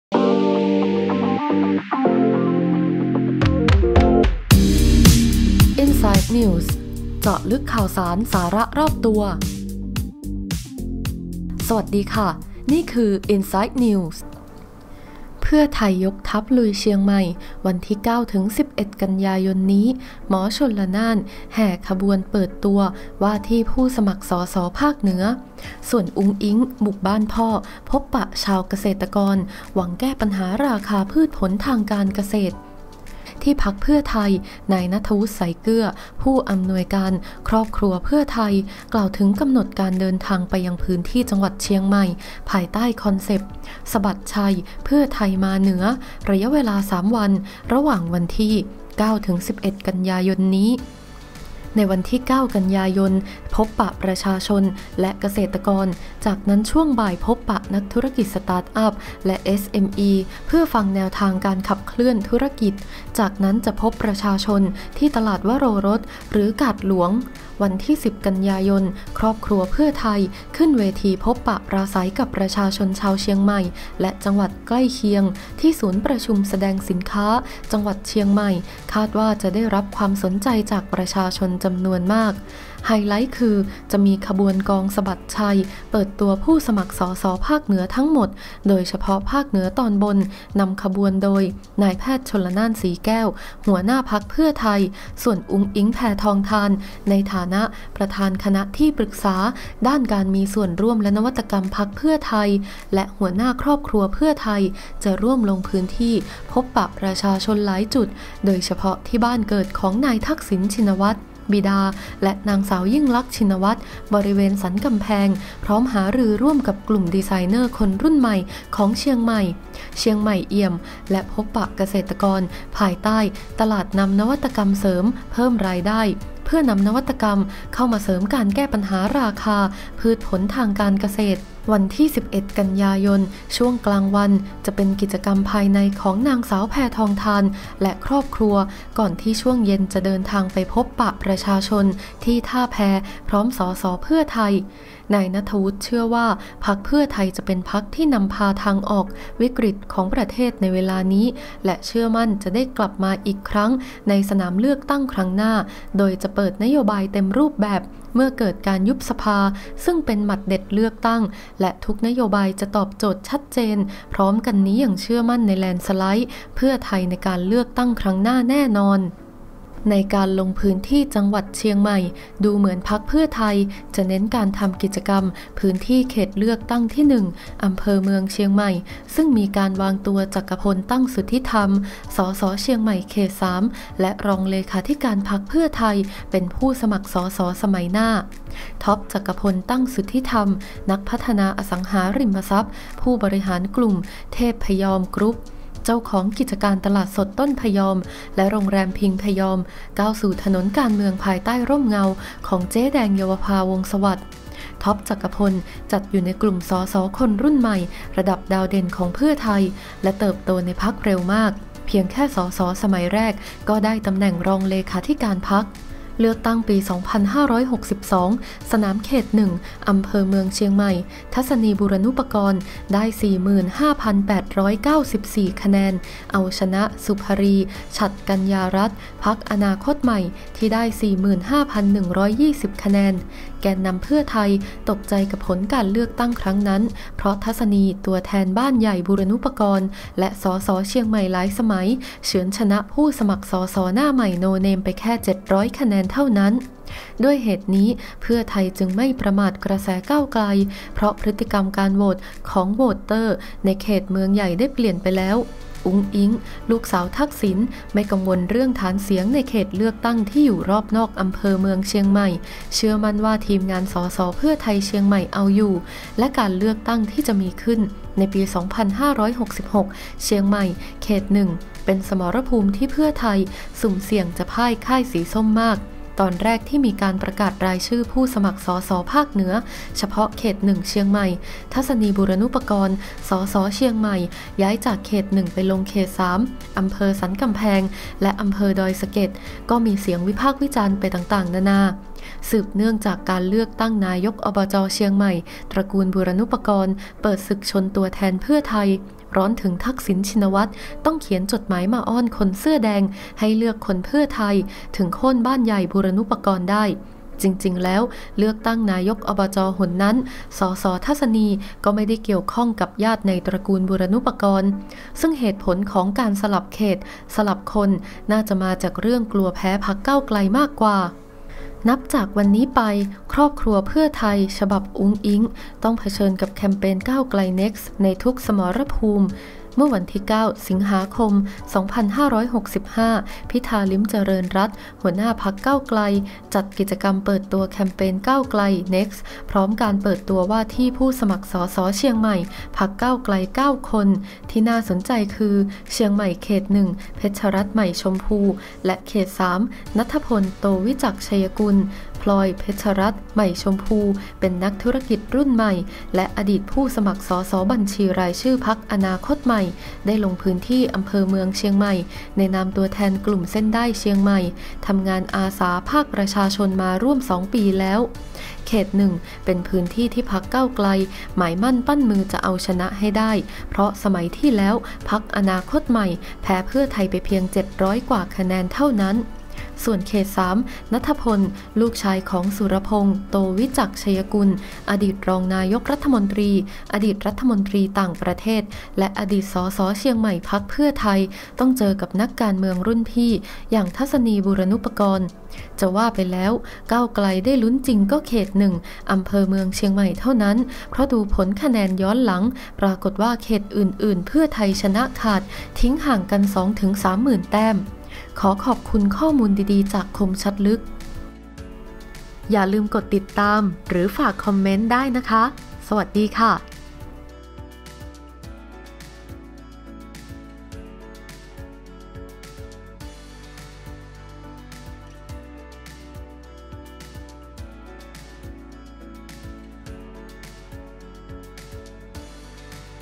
"เพื่อไทย" ยกทัพลุยเชียงใหม่ "อุ๊งอิ๊ง" บุกบ้านพ่อ พบชาวเกษตรกร รักษาที่มั่น พื้นที่อันตราย
"เพื่อไทย" ยกทัพลุยเชียงใหม่ วันที่ 9-11 ก.ย.นี้ "หมอชลน่าน" แห่ขบวนเปิดตัว ว่าที่ผู้สมัคร ส.ส. ภาคเหนือ ส่วน"อุ๊งอิ๊ง" บุกบ้านพ่อ พบปะชาวบ้านเกษตรกร หวังแก้ปัญหาราคาพืชผลทางการเกษตร
“อุ๊งอิ๊ง” ยกทัพไปบ้านเกิดพ่อแม้ว พิทักษ์ที่มั่นเขต 1 เหตุ "ก้าวไกล"มาแรง หากเชียงใหม่ไม่ชนะยกจังหวัด เพื่อไทยแลนด์สไลด์ทั้งภาคเหนือ ก็ไร้ความหมาย

ที่ พรรคเพื่อไทย นายณัฐวุฒิ ใสยเกื้อ ผู้อำนวยการ ครอบครัวเพื่อไทย กล่าวถึงกำหนดการเดินทางไปพื้นที่ จังหวัดเชียงใหม่ ภายใต้คอนเซ็ป "สะบัดชัย เพื่อไทยมาเหนือ" ระยะเวลา 3 วัน ระหว่างวันที่ 9-11 กันยายนนี้

วันที่ 9 กันยายน พบปะประชาชน และเกษตรกร จากนั้นช่วงบ่ายพบปะนักธุรกิจสตาร์ทอัพ และ เอสเอ็มอี เพื่อฟังแนวทางการขับเคลื่อนธุรกิจ จากนั้นจะพบประชาชนที่ ตลาดวโรรส หรือ กาดหลวง

วันที่ 10 กันยายน ครอบครัวเพื่อไทย ขึ้นเวทีพบปะปราศรัยกับประชาชนชาวเชียงใหม่และจังหวัดใกล้เคียงที่ศูนย์ประชุมแสดงสินค้าจังหวัดเชียงใหม่ คาดว่าจะได้รับความสนในจากประชาชนจำนวนมาก
ไฮไลท์ คือ จะมีขบวนกองสะบัดชัย เปิดตัวผู้สมัคร ส.ส. ภาคเหนือ ทั้งหมด โดยเฉพาะภาคเหนือตอนบน นำขบวนโดย นายแพทย์ ชลน่าน ศรีแก้ว หัวหน้าพรรคเพื่อไทย ส่วน อุ๊งอิ๊ง แพทองธาร ในฐานะประธานคณะที่ปรึกษาด้านการมีส่วนร่วมและนวัตกรรมพรรคเพื่อไทย และหัวหน้าครอบครัวเพื่อไทย จะร่วมลงพื้นที่พบปะประชาชนหลายจุด โดยเฉพาะที่บ้านเกิดของนายทักษิณ ชินวัตร บิดา และ นางสาวยิ่งลักษณ์ ชินวัตร บริเวณสันกำแพง พร้อมหารือร่วมกับกลุ่มดีไซเนอร์คนรุ่นใหม่ของเชียงใหม่ "เชียงใหม่เอี่ยม" และ พบปะเกษตรกร ภายใต้ "ตลาดนำ นวัตกรรมเสริม เพิ่มรายได้" เพื่อนำนวัตกรรมเข้ามาเสริมการแก้ปัญหาราคา พืชผลทางเกษตร

วันที่ 11 กันยายน ช่วงกลางวันจะเป็นกิจกรรมภายในของนางสาวแพทองธาร และครอบครัว ก่อนที่ช่วงเย็นจะเดินพบประชาชนที่ท่าแพ พร้อม ส.ส.เพื่อไทย

นายณัฐวุฒิ เชื่อว่า พรรคเพื่อไทยจะเป็นพรรคที่นำพาทางออกวิกฤตของประเทศในเวลานี้ และเชื่อมั่นจะได้กลับมาอีกครั้งในสนามเลือกตั้งครั้งหน้า โดยจะเปิดนโยบายเต็มรูปแบบ เมื่อเกิดการยุบสภา ซึ่งเป็นหมัดเด็ดเลือกตั้ง และทุกนโยบายจะตอบโจทย์อย่างชัดเจน พร้อมกันนี้ยังเชื่อมั่น แลนด์สไลด์เพื่อไทย ในการเลือกตั้งครั้งหน้าแน่นอน

ดูเหมือนพรรคเพื่อไทย จะเน้นการทำกิจกรรมในพื้นที่เขตเลือกตั้งที่ 1 อ.เมืองเชียงใหม่ ซึ่งมีการวางตัว จักรพล ตั้งสุทธิธรรม ส.ส.เชียงใหม่ เขต 3 และรองเลขาธิการพรรคเพื่อไทย เป็นผู้สมัคร ส.ส.สมัยหน้า
ท็อป-จักรพล ตั้งสุทธิธรรม นักพัฒนาอสังหาริมทรัพย์ ผู้บริหารกลุ่มเทพพยอมกรุ๊ป, เจ้าของกิจการตลาดสดต้นพยอม และโรงแรมพิงค์พยอม ก้าวสู่ถนนการเมืองภายใต้ร่มเงาของเจ๊แดง-เยาวภา วงศ์สวัสดิ์
ท็อป จักรพล จัดอยู่ในกลุ่ม ส.ส.คนรุ่นใหม่ระดับดาวเด่นของเพื่อไทย และเติบโตในพรรคเร็วมาก เพียงแค่ ส.ส.สมัยแรก ก็ได้ตำแหน่งรองเลขาธิการพรรค

เลือกตั้งปี 2562 สนามเขต 1 (อ.เมืองเชียงใหม่) ทัศนีย์ บูรณุปกรณ์ ได้45,894 คะแนน เอาชนะศุภรี ฉัตรกันยารัตน์ พรรคอนาคตใหม่ ที่ได้ 45,120 คะแนน
แกนนำเพื่อไทย ตกใจกับผลการเลือกตั้งครั้งนั้น เพราะทัศนีย์ ตัวแทนบ้านใหญ่บูรณุปกรณ์ และ ส.ส.เชียงใหม่หลายสมัย เฉือนชนะผู้สมัคร ส.ส.หน้าใหม่โนเนมไปแค่ 700 คะแนนเท่านั้น
ด้วยเหตุนี้ เพื่อไทยจึงไม่ประมาทกระแสก้าวไกล เพราะพฤติกรรมของโหวตเตอร์ในเขตเมืองใหญ่ได้เปลี่ยนไปแล้ว

เมื่อวันที่ 9 ส.ค.2565 พิธา ลิ้มเจริญรัตน์ หัวหน้าพรรคก้าวไกล ได้จัดกิจกรรมเปิดตัวแคมเปญก้าวไกล Next พร้อมการเปิดตัวว่าที่ผู้สมัคร ส.ส.เชียงใหม่ พรรคก้าวไกล 9 คน
ที่น่าสนใจคือ เชียงใหม่ เขต 1 เพชรรัตน์ ใหม่ชมภู และเขต 3 ณัฐพล โตวิจักษณ์ชัยกุล   พลอย-เพชรรัตน์ ใหม่ชมภู นักธุรกิจรุ่นใหม่ และอดีตผู้สมัคร ส.ส.บัญชีรายชื่อพรรคอนาคตใหม่ ได้ลงพื้นที่ อ.เมืองเชียงใหม่ ในนามตัวแทนกลุ่มเส้นด้าย เชียงใหม่ ทำงานอาสาภาคประชาชนมาร่วม 2 ปีแล้ว
เขต 1 เป็นพื้นที่ที่พรรคก้าวไกลหมายมั่นปั้นมือจะเอาชนะให้ได้ เพราะสมัยที่แล้ว พรรคอนาคตใหม่ แพ้เพื่อไทยไปเพียง 700 กว่าคะแนนเท่านั้น
ส่วนเขต 3 ณัฐพล ลูกชายของสุรพงษ์ โตวิจักษณ์ชัยกุล อดีตรองนายกรัฐมนตรี อดีตรัฐมนตรีต่างประเทศ และอดีต ส.ส.เชียงใหม่ พรรคเพื่อไทย ต้องเจอกับนักการเมืองรุ่นพี่อย่าง ทัศนีย์ บูรณุปกรณ์
จะว่าไปแล้ว ก้าวไกลได้ลุ้นจริงๆก็เขต 1 อ.เมืองเชียงใหม่เท่านั้น เพราะดูผลคะแนนย้อนหลัง ปรากฏว่า เขตอื่นๆ เพื่อไทยชนะขาด ทิ้งห่างกัน 2-3 หมื่นแต้ม

คมชัดลึก komchadluek